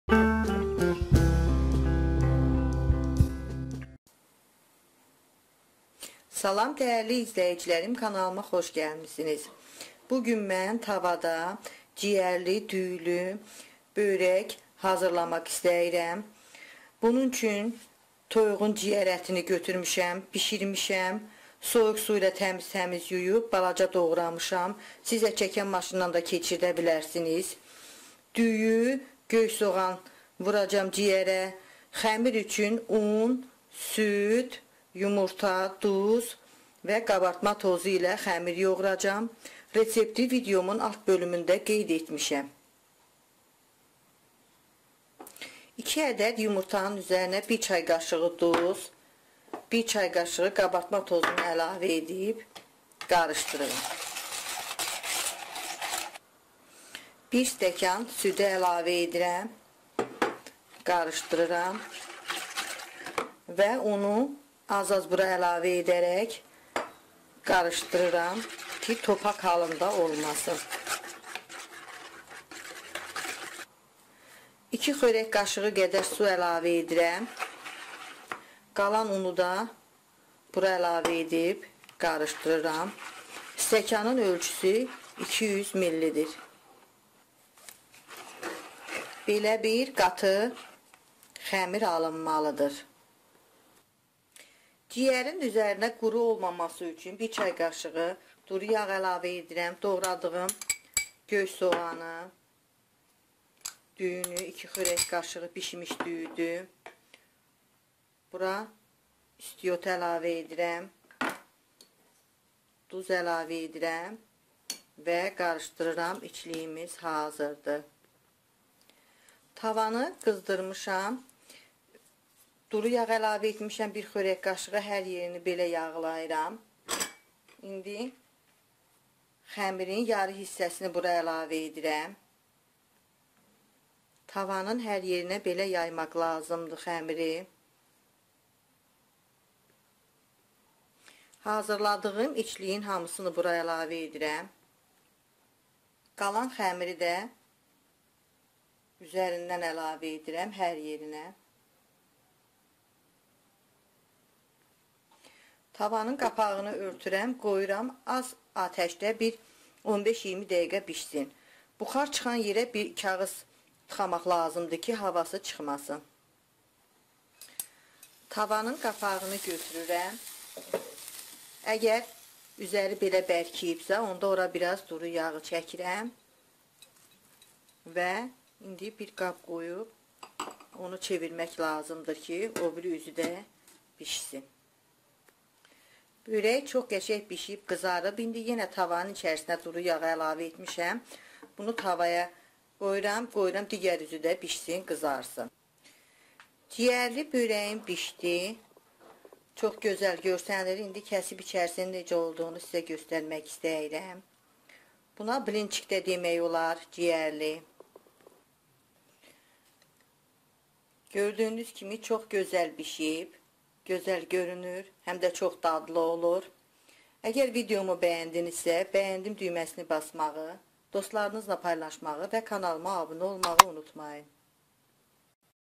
Tavada dadli ciyərli, düyülü Börək .Tavada cigerli,pirincli borek.
Ərzaqlar.
2 st -un (200ml )
2 ədəd -yumurta
1 st -süd (st 250ml)
1ç.q -duz
1ç.q -qabartma tozu
2x.q -su
içlik.
300q-toyuq ciyəri bişmiş
2x.q -düyü bişmiş
2 qanad -göy sogan
1ç.q -duru yag
duz istiot
tavani yaglamaq üçün-3-4 x.q -duru yag